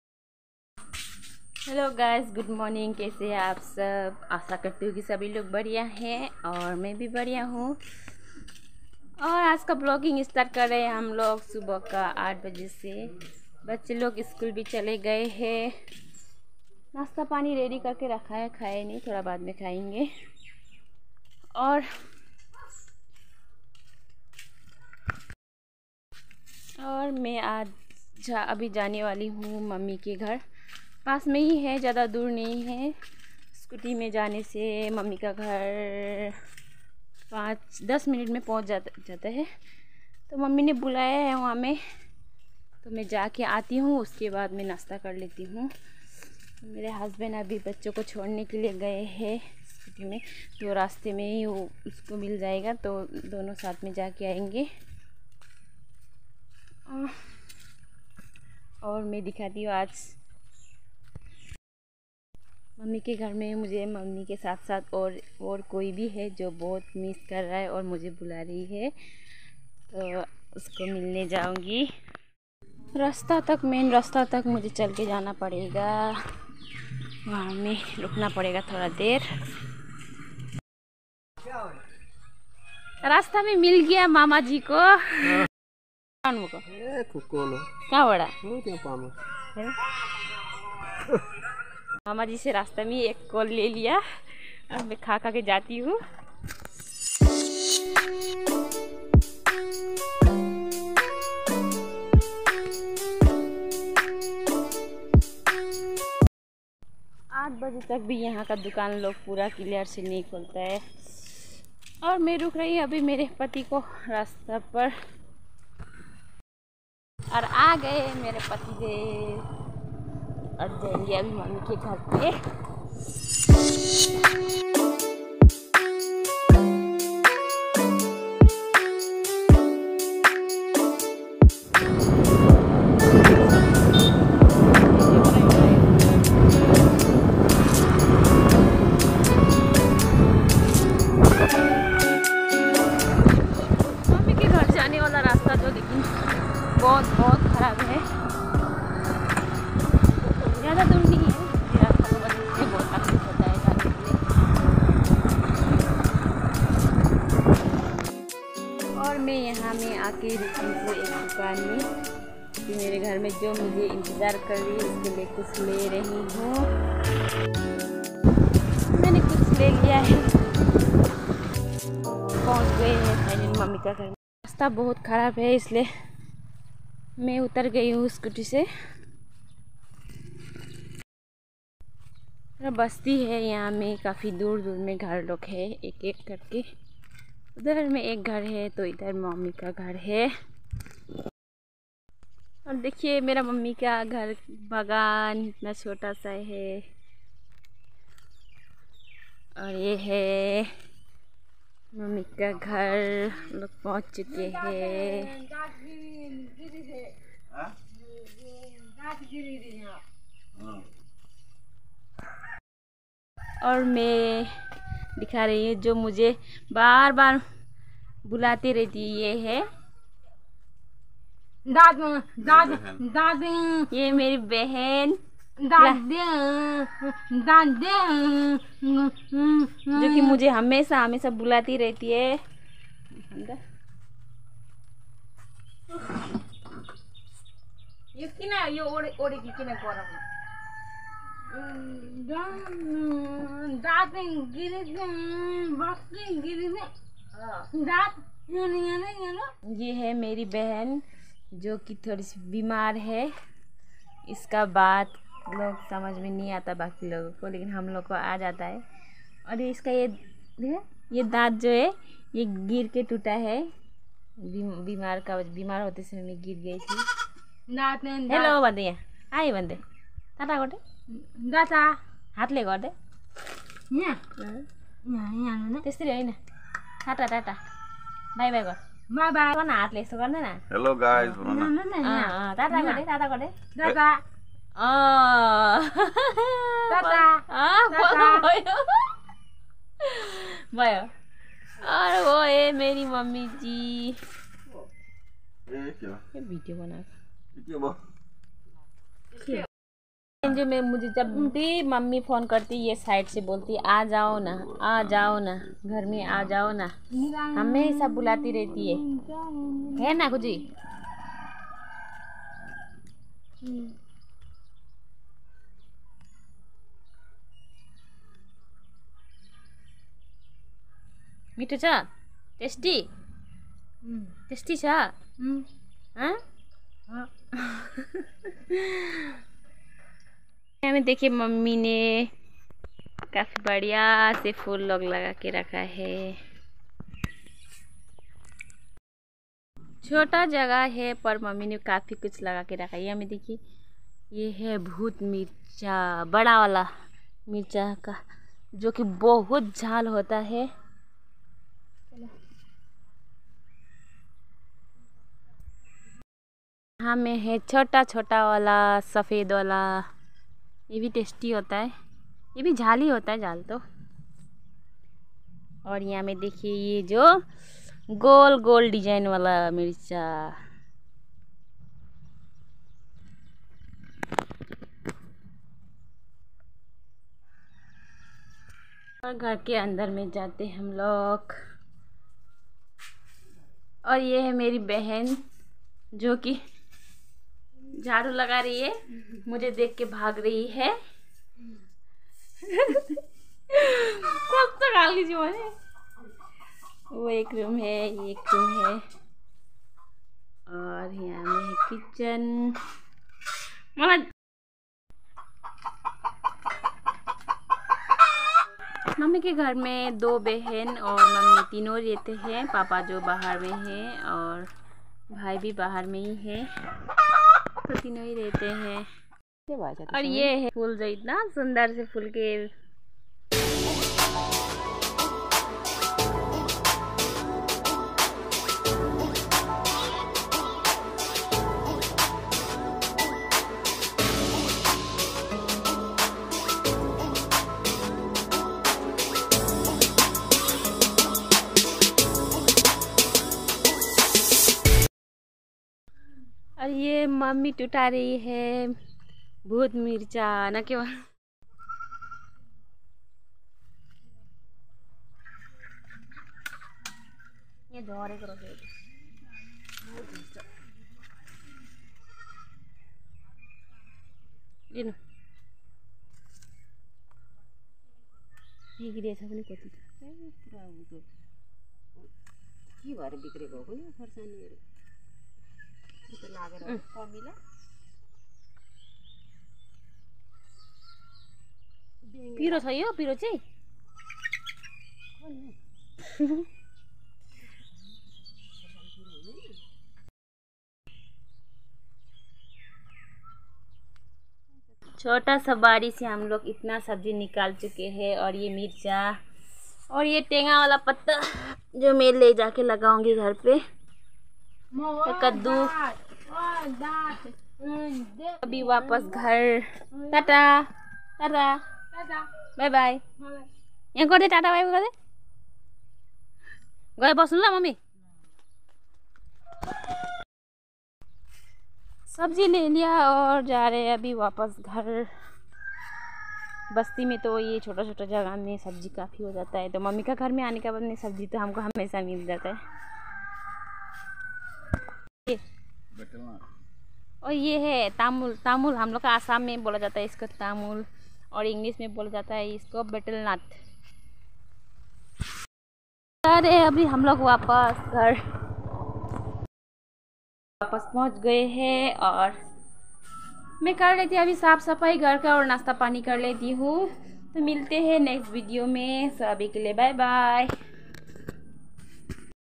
youtube video# mammy ke ghar🏠 koi mera intejar kar rahi hai..
##Aaj me mammy ke ghar gyi thi waha meri bahan mera intejar kar rahi thi ##